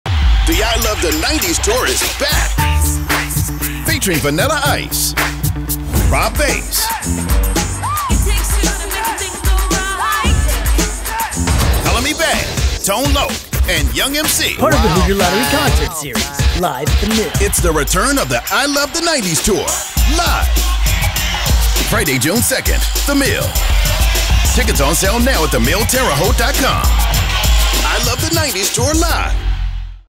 The Hoosier Lottery Concert Series Presents: I Love the 90's Tour
Vanilla Ice, Rob Base, Color Me Badd, Tone Loc and Young MC are headed to The Mill on June 2nd!